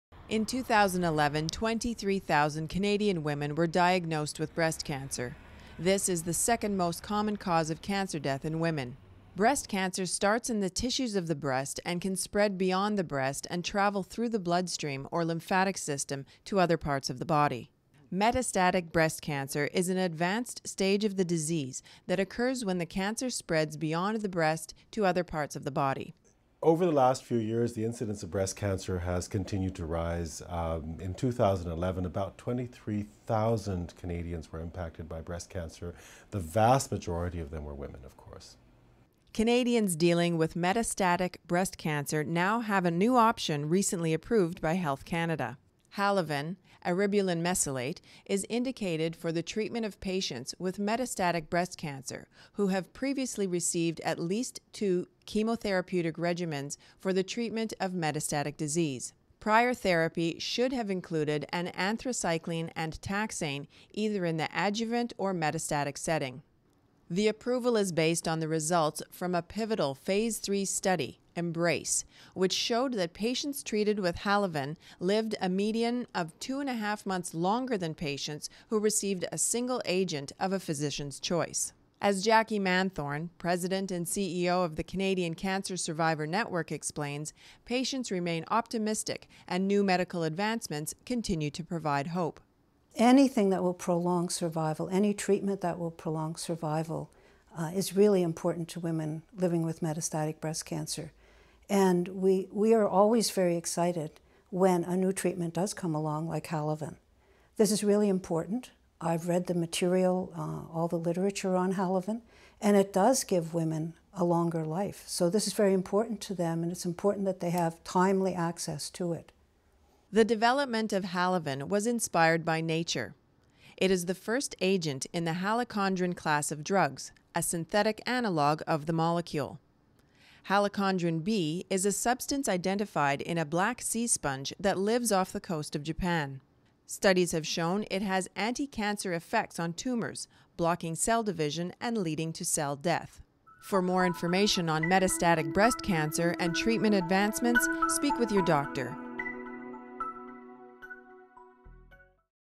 New Treatment Option for Breast Cancer Halaven™
New Treatment Option Halaven™, Significantly Prolongs Overall Survival in Pre-treated Metastatic Breast Cancer Patients.